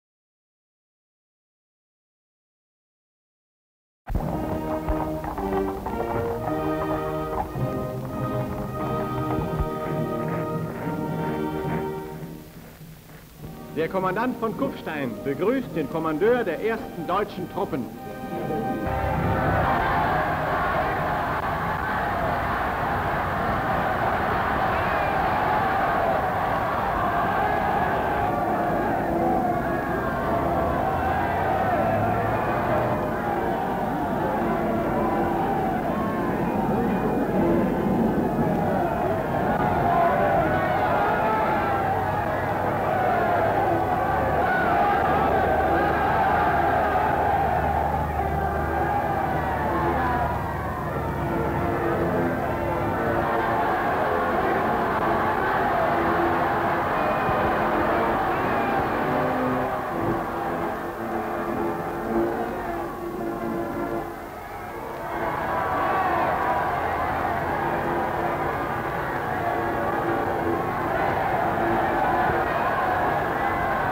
The Anschluss (Annexation of Austria by Germany) in 1938. German troops enter Kuf...HD Stock Footage
The Anschluss (Annexation of Austria by Germany) in 1938. German troops enter Kufstein, Austria.

The Anschluss (Annexation of Austria by Germany) in 1938. Sign reads 'Bundesstaat Oesterreich' (State of Austria). Mounted German troops ride along the platform of railroad station. Sign reads: "Staatsgrenze" (State Border). Mayor of Kufstein greets mounted Commander of German troops as they cross the border between Germany and Austria. View of Kufstein castle. Crowds cheer as German troops proceed through the town. Location: Kufstein Austria. Date:  1938.

57,000+ broadcast-quality historic clips for immediate download.